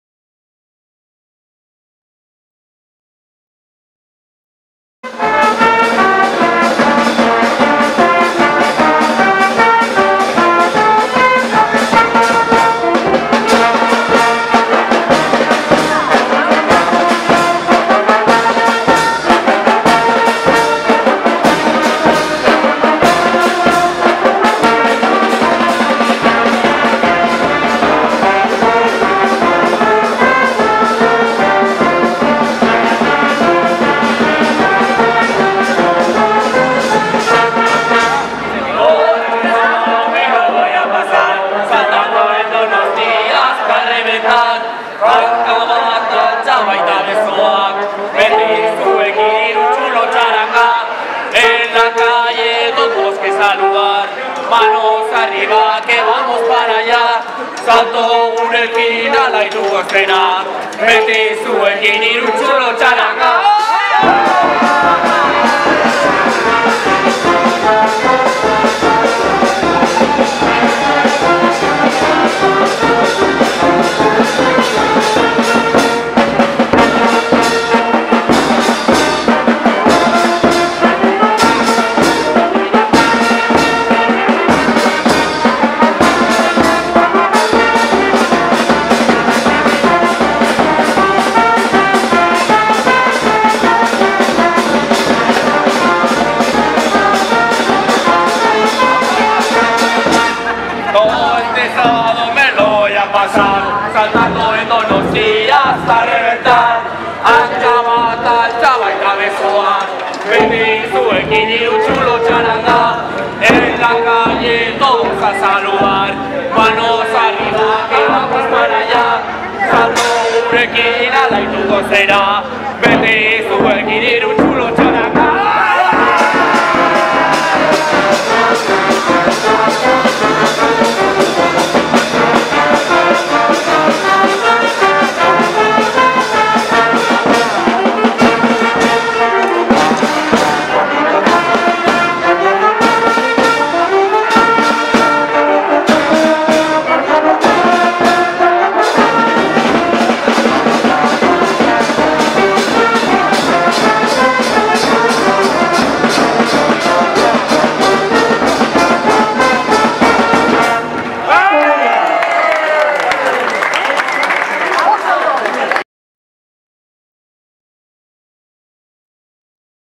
Inauteri festak ez du adinik
Erritmo bizian dantzatu da jendea Donostiako 3txulo txarangaren erritmoan, Parte Zaharrean.